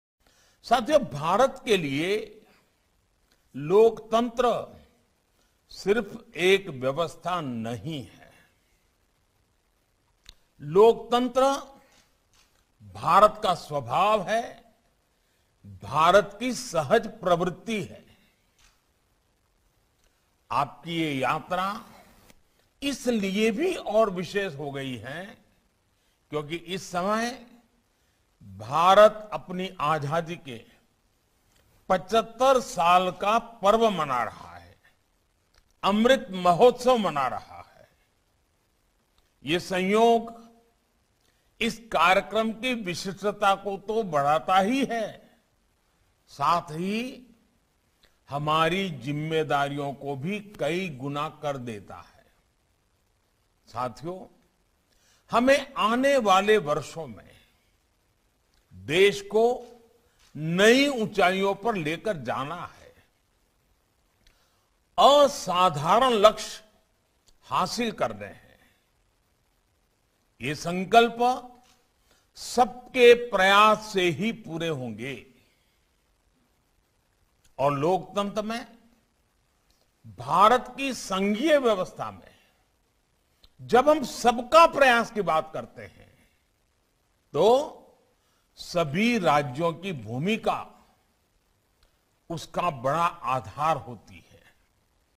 PM Narendra Modi addresses the inaugural session of 82nd All India Presiding Officers’ Conference
Prime Minister Narendra Modi addresses the inaugural session of 82nd All India Presiding Officers’ Conference

He said, "भारत के लिए लोकतंत्र सिर्फ एक व्यवस्था नहीं है। लोकतंत्र भारत का स्वभाव है, भारत की सहज प्रवृति है।"